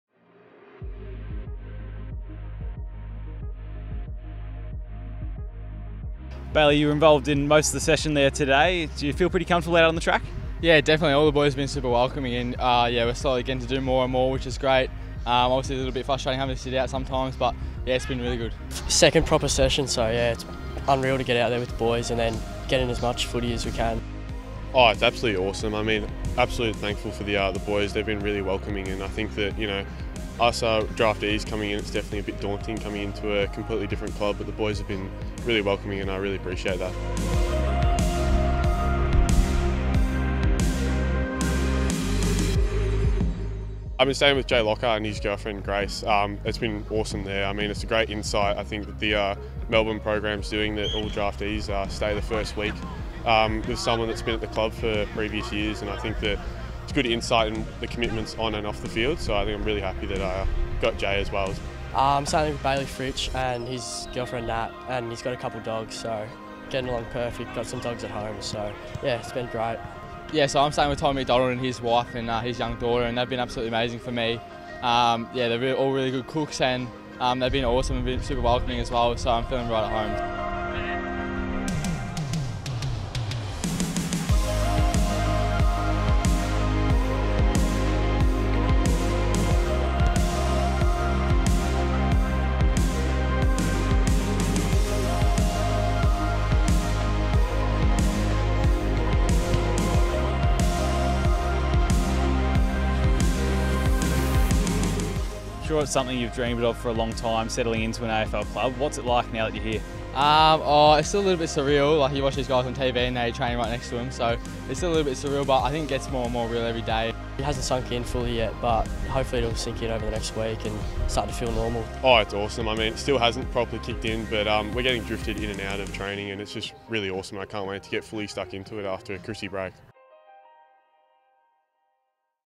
Draftees | On the Track
Melbourne's new draftees head to Casey Fields for their first week of training.